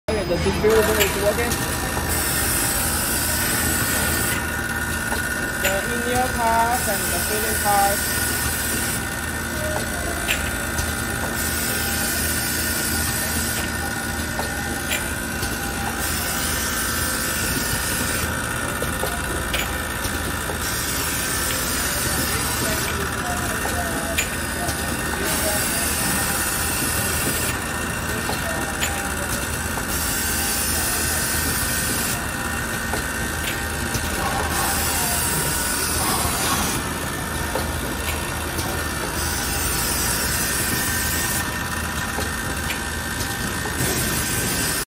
automatic reed diffuser liquid filling machine
multifunctional model, integrating filling, plugging and capping